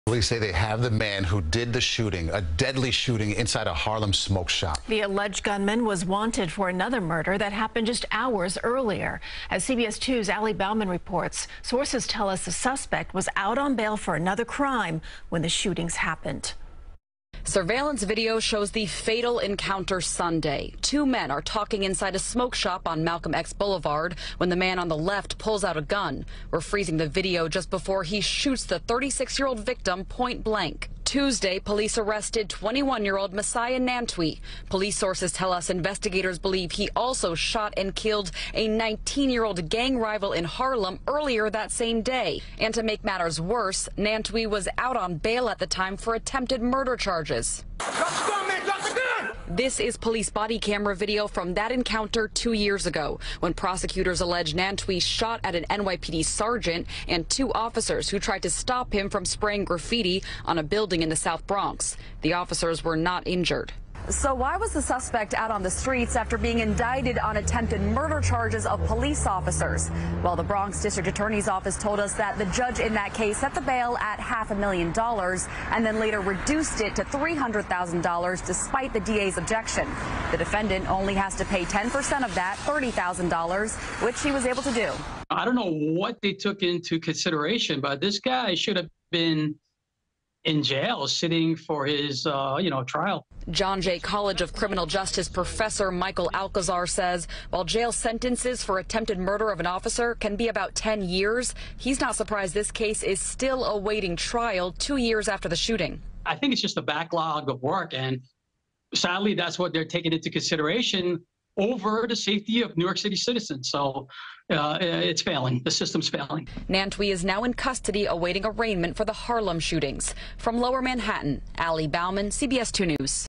Arrest made in Harlem smoke shop fatal shooting
CBS2's Ali Bauman has learned the suspect was wanted for another fatal shooting an hour earlier.